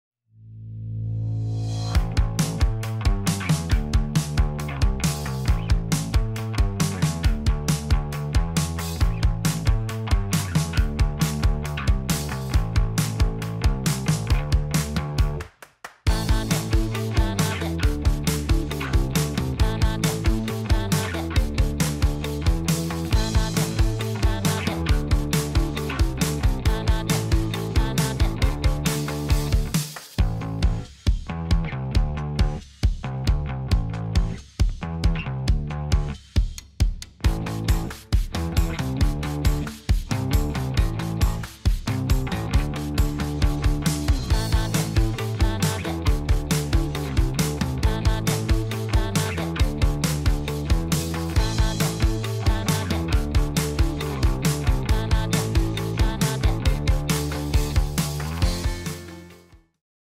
Forester Wilderness 2022 - Voyez-en plus, découvrez-en plus et vivez-en plus.
Voici la toute première Subaru Forester Wilderness, la Forester la plus solidement compétente jamais conçue. C'est la Forester que vous connaissez et appréciez déjà, parée des mêmes atouts de qualité, de fiabilité et de durabilité. Mais cette Forester est aussi radicalement différente : c’est un VUS compact conçu pour vous encourager à en faire plus, en voir plus et en découvrir plus.